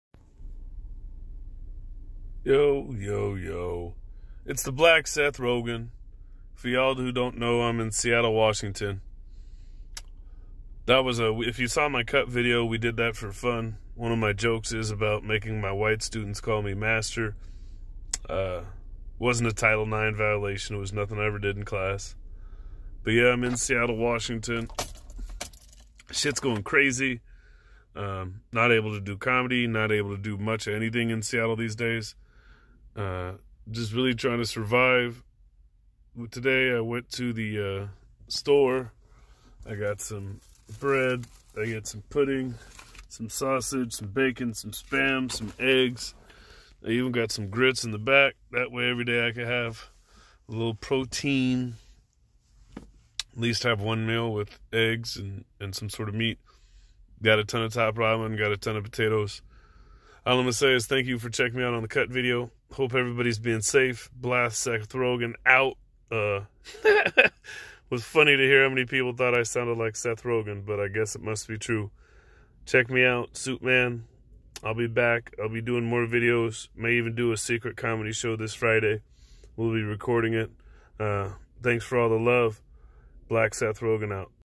SuitMan BlackSethRogan Quarantined
Shout out to all new followers... Stuck indoors in Seattle. Grabbed some essentials to get through.